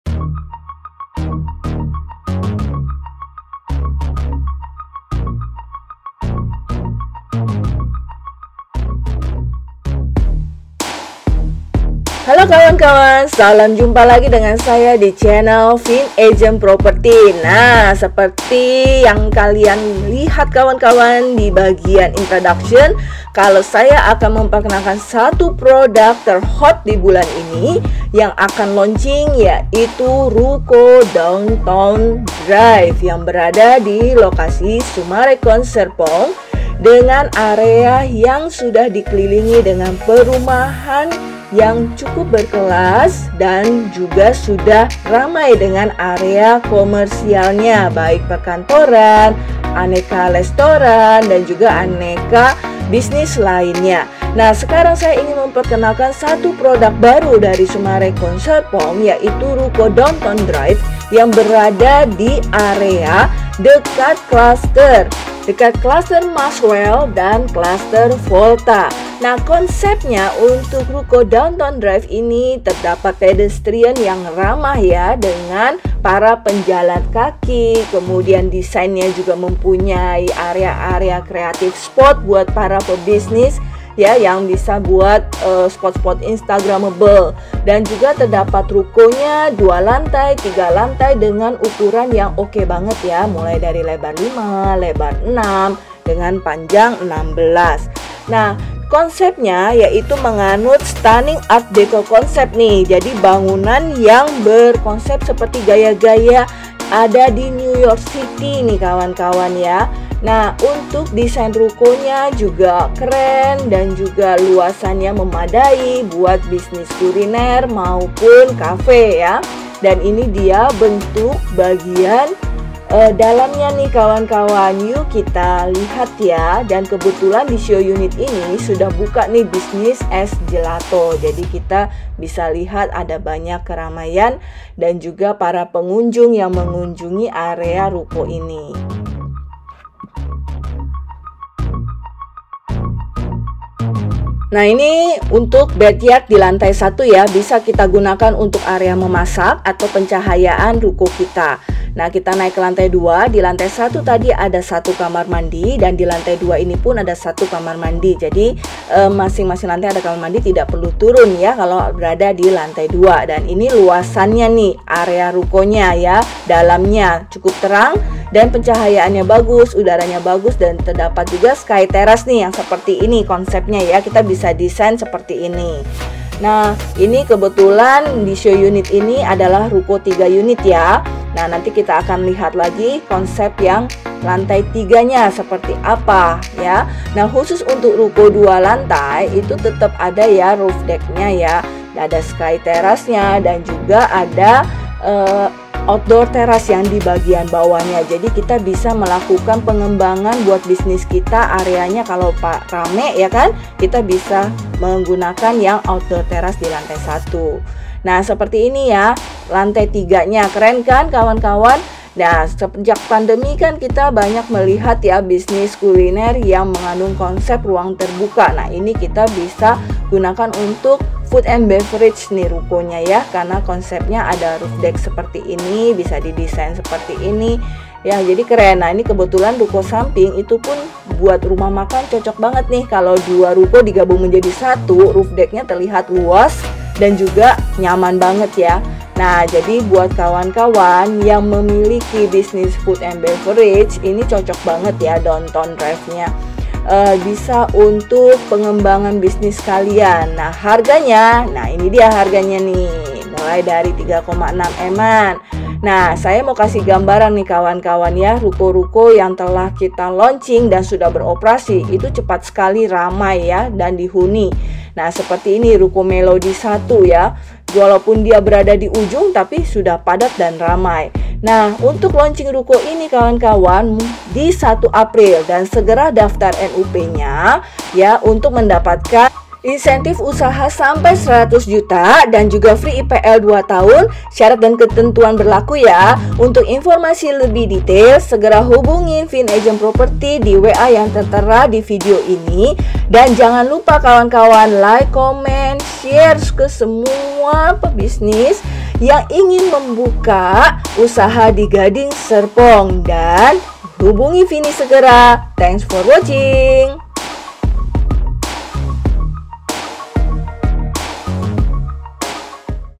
RUKO DOWNTOWN DRIVE SUMMARECON SERPONG, KEREN SKY TERRACE & ROOF DECK, COCOK BUAT F&B, YUKS DIBELI
RUKO DOWNTOWN DRIVE SUMMARECON SERPONG, KEREN  SKY TERRACE & ROOF DECK, COCOK BUAT F&B, YUKS DIBELI HUB  LANGSUNG FIN DI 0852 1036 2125, TERSEDIA RUKO 2 LANTAI DAN 3 LANTAI DENGAN KONSEP ART DECO RUKO RUKO ALA DI NEW YORK GITU LOH GUYS.. COCOK BANGET BUAT PEBISNIS MATURE DAN KAWULA MUDA . NGA BAKAL RUGI DEH BELI RUKO INI  BAIK BUAT BISNIS MAUPUN INVESTASI, LOKASI DAN LINGKUNGAN MENUNJANG BANGET LOH BUAT BISNIS, UKURAN RUKO ADA L5 DAN L6 DENGAN PANJANG 16 HARGA MULAI 3MAN DENGAN CARA BAYAR BERVARIASI DAN DUKUNGAN KPR BANK YANG OKE OKE .. CUZZ BELI AMA FIN  DI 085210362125 SEGERA DEH DAFTAR PEMBELIAANNYA SEBELUM 22 MARET BIAR DAPAT KEUNTUNGAN LAGI NIH BERUPA IPL FREE 2 THN DAN INSENTIF USAHA SAMPAI 100JUTA ( SYARAT DAN KETENTUAN BERLAKU YA )